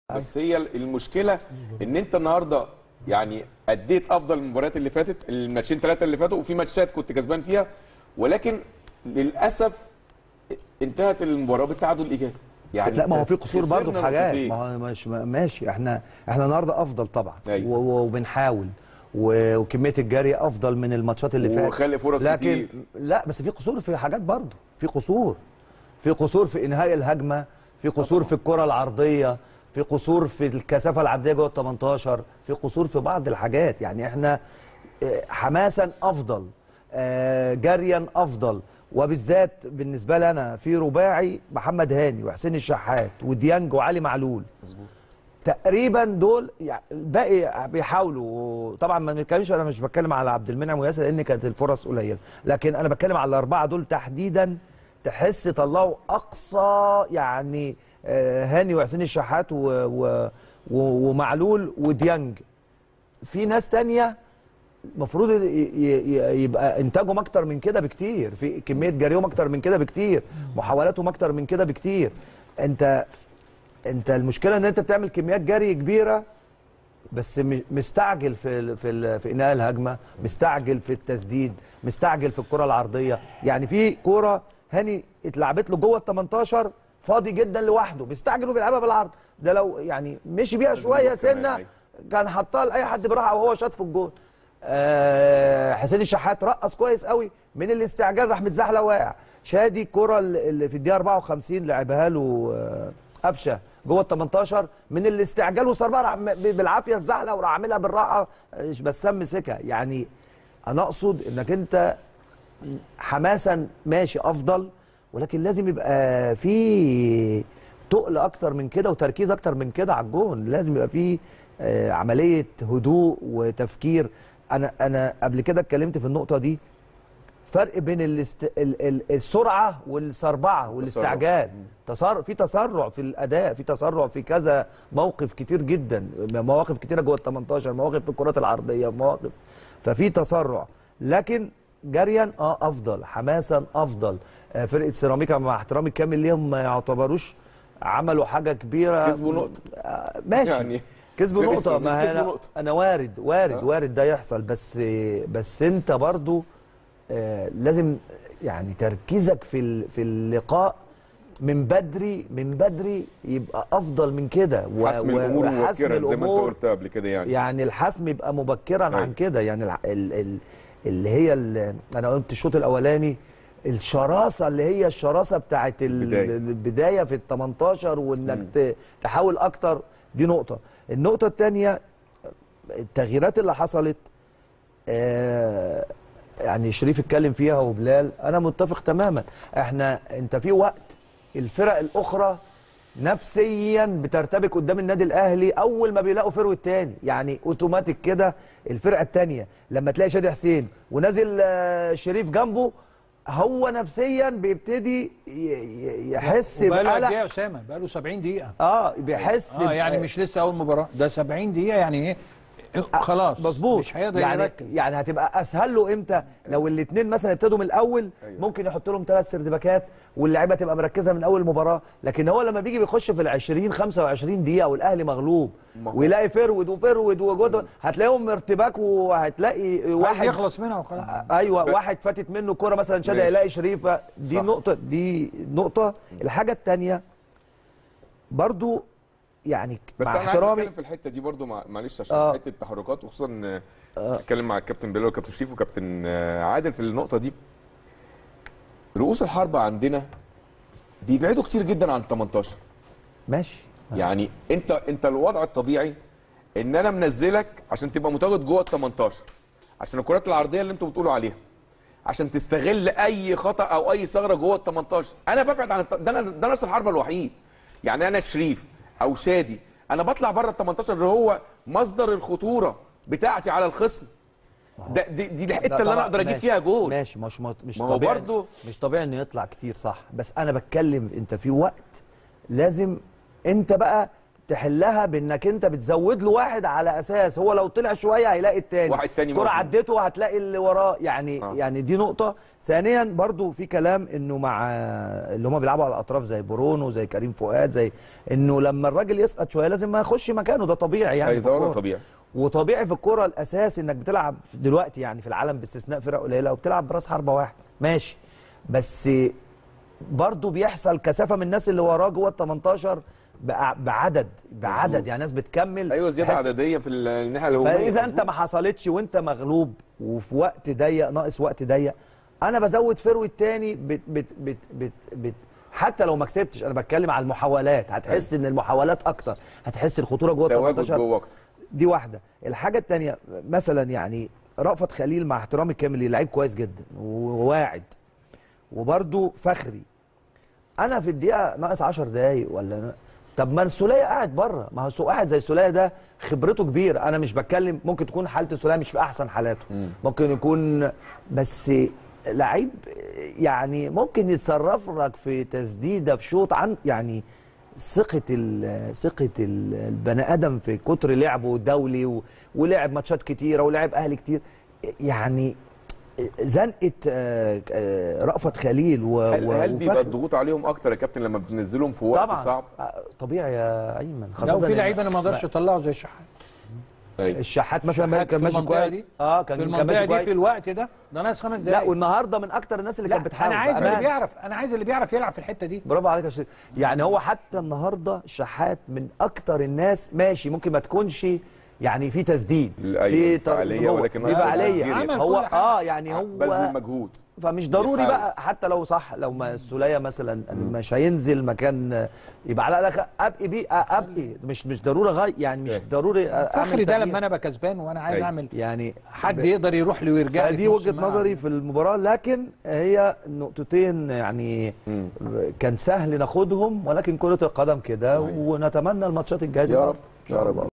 أسامة عرابي: مستوى الأهلي أفضل من المباريات السابقة ولكن هناك قصور في بعض الجوانب.
أسامة عرابي: مستوى الأهلي أفضل من المباريات السابقة ولكن هناك قصور في بعض الجوانب | الاستوديو التحليلي